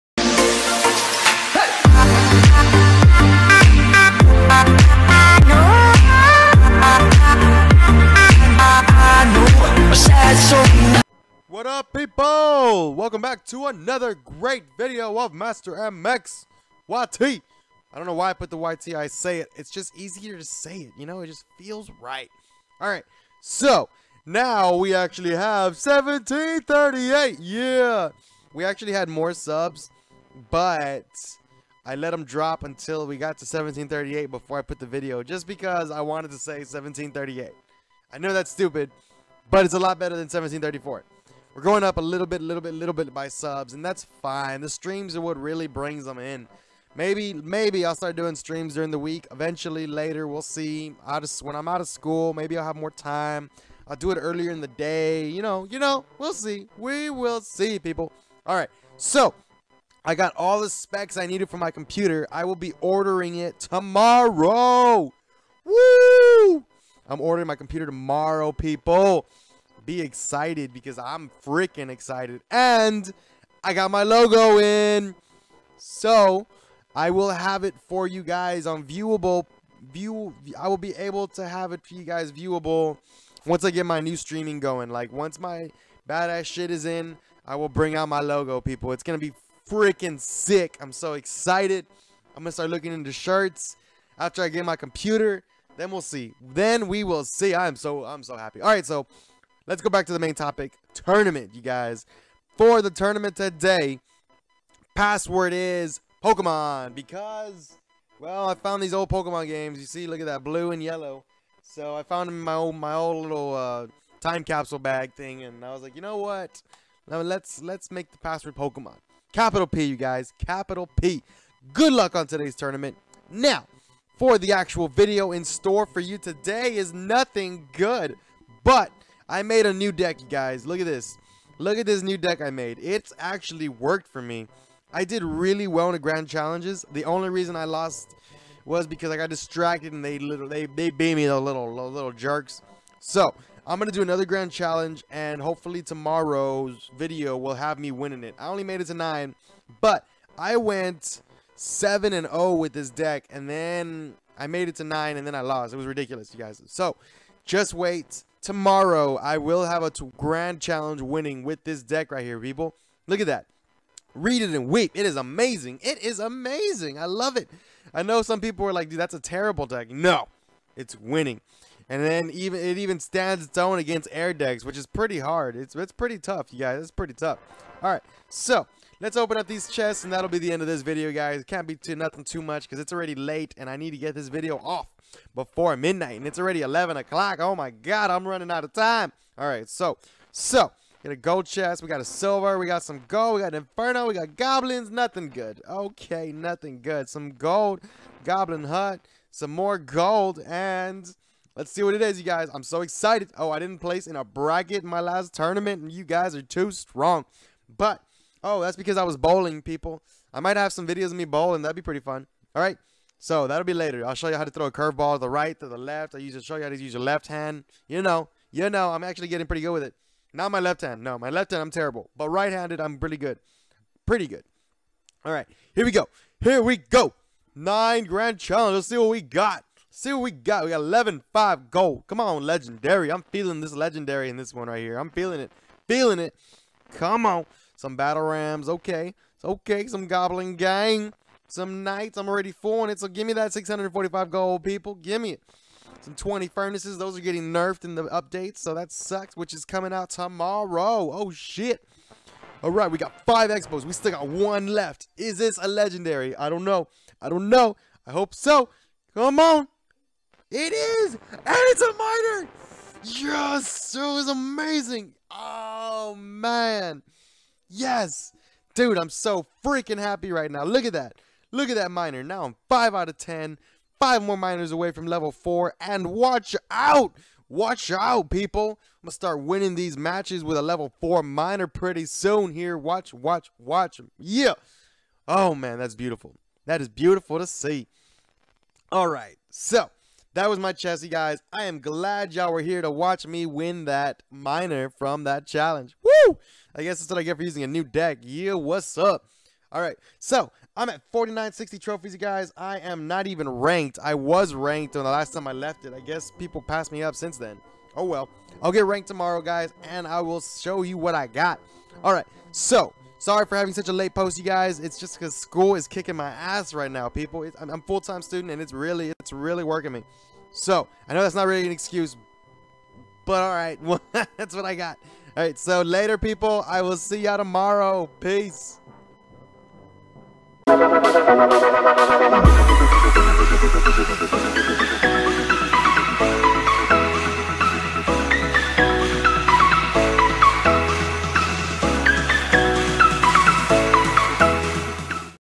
500 Gem Tourny Password and a Awsome new grand challenge deck
Alright just another fun quick video goodluck everyone see you guys on the battlefield